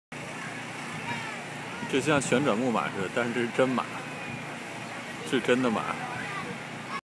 Carousel with real ponies
At Berkeley kite fest 2011. This is my first YouTube upload. This video is mainly for testing how to upload a video from a iPhone. In the video I was saying, "this is like a carousel but with real ponies, real ponies!" Poor ponies.